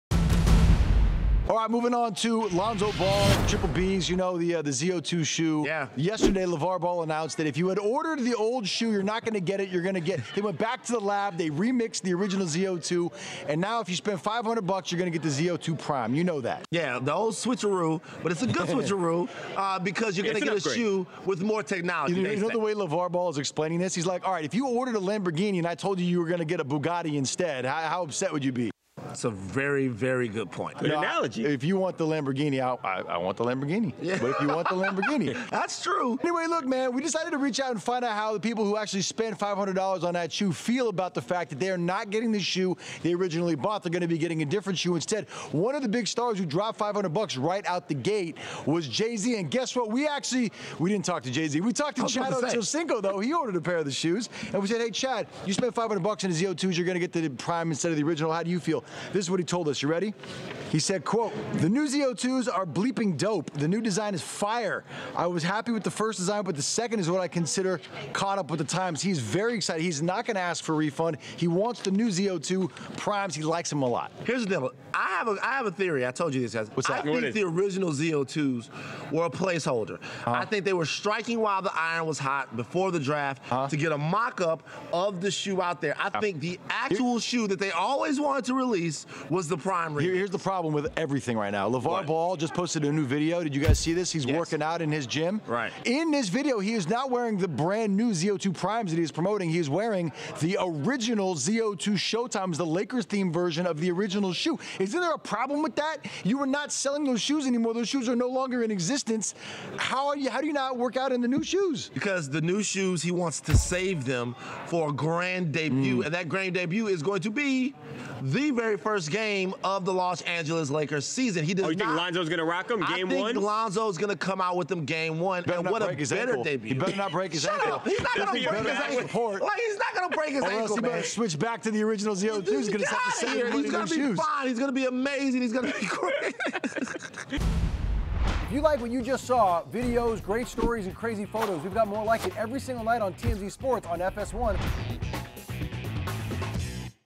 Chad Ochocinco: New ZO2's Are 'F**king Dope,' Excited To Get Mine! | TMZ Sports
Chad Ochocinco: New ZO2's Are 'F**king Dope,' Excited To Get Mine!